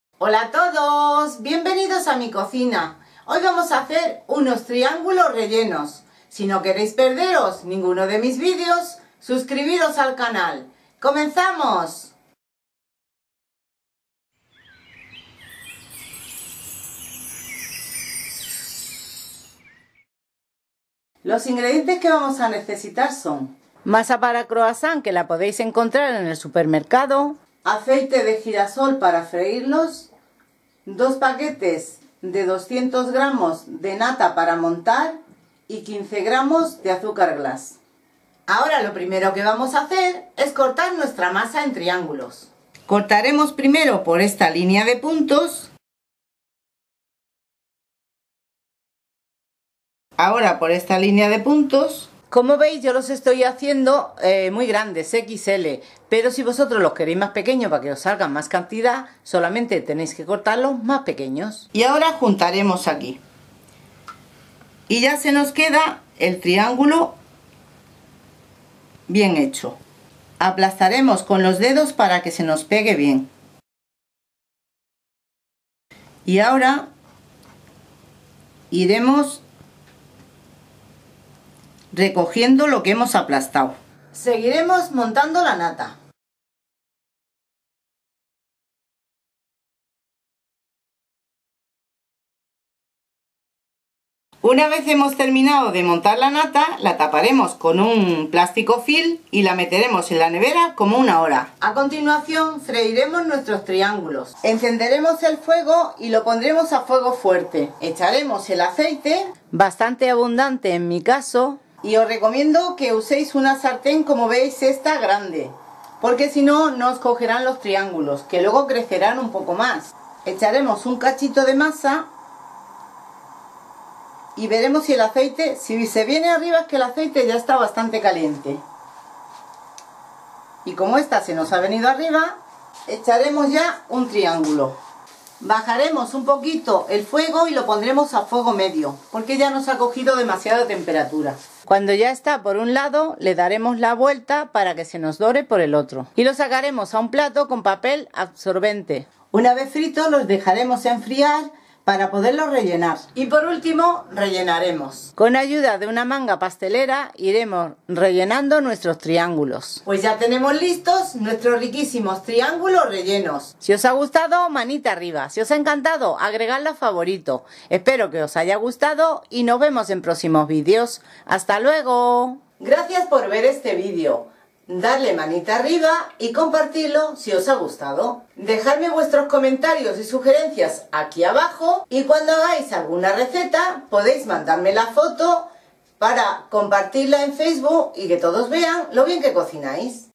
Triángulos rellenos de nata | Recetas de cocina | Cocina de Martina
En esta ocasión, os enseño cómo hacer unos riquísimos triángulos rellenos de nata
Comenta y dale a "Like" si te ha gustado! Si te ha encantado ponlo en Favoritos!! Muchas Gracias!!!